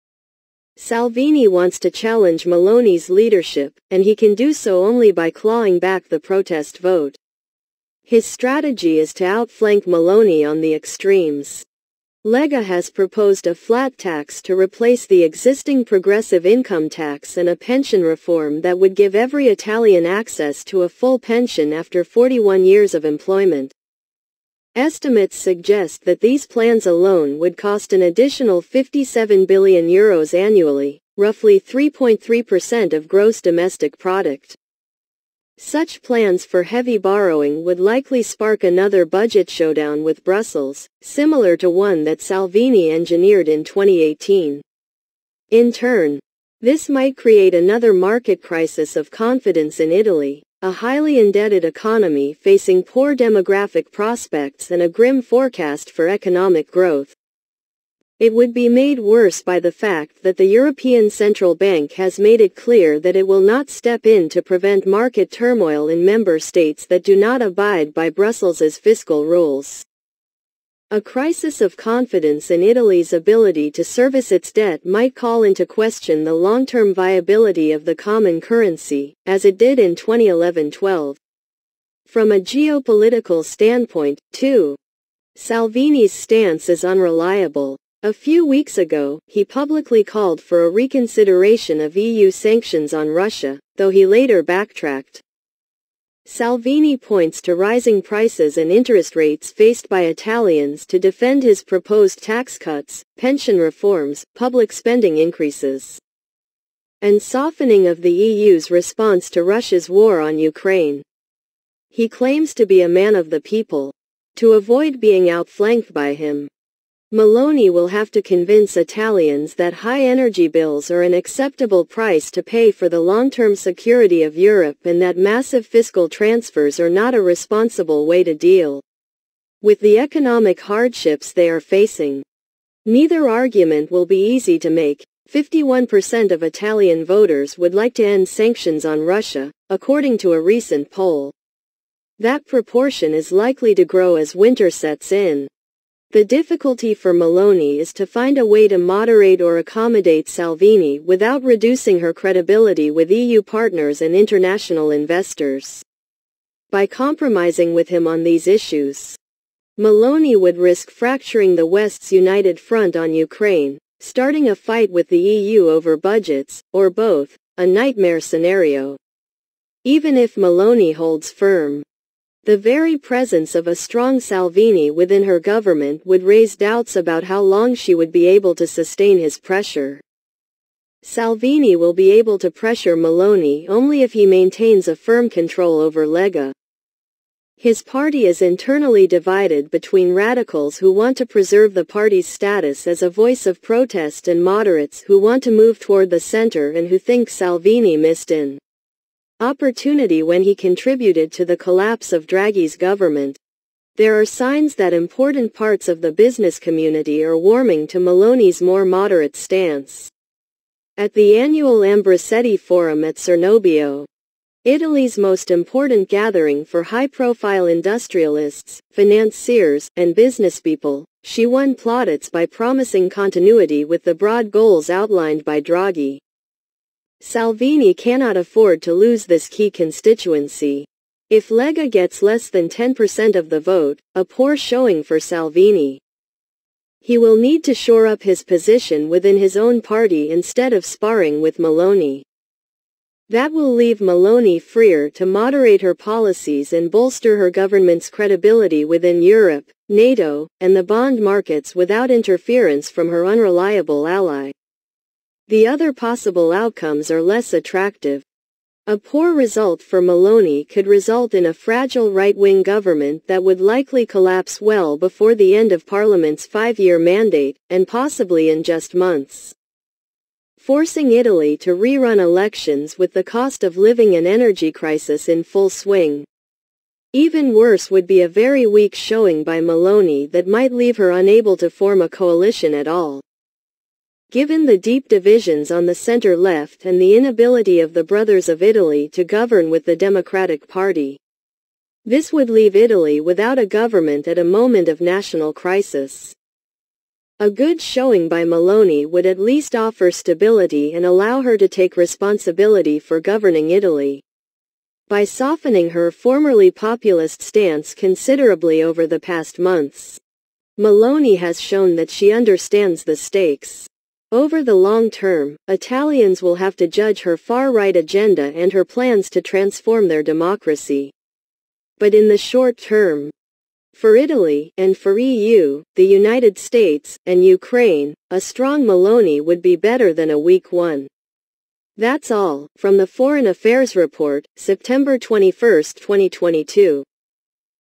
Italy’s Election Paradox (2/2)
Why America and the EU Should Root for a Far-Right Populist.
We would like to share the Foreign Affairs report, September 21, 2022.